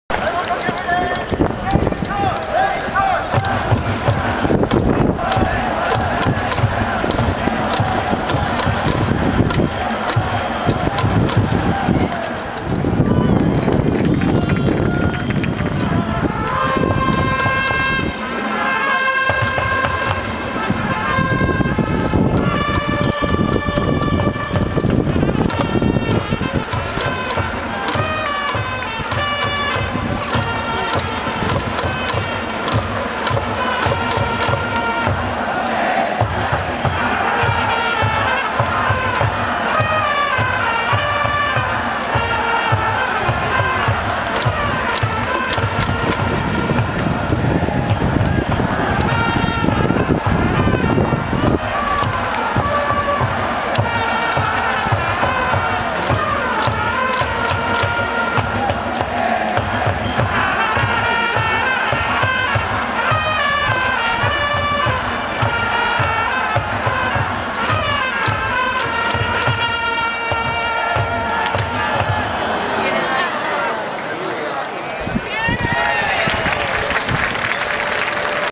【2024 オープン戦】広島カープ ジェイク・シャイナーのバッティングフォーム_2024030202
2024年オープン戦、東北楽天ゴールデンイーグルス VS 広島東洋カープ@倉敷マスカットスタジアムでの広島カープ ジェイク・シャイナーのバッティングフォームです。(遠くからの撮影でブレが大きめです。すみません。)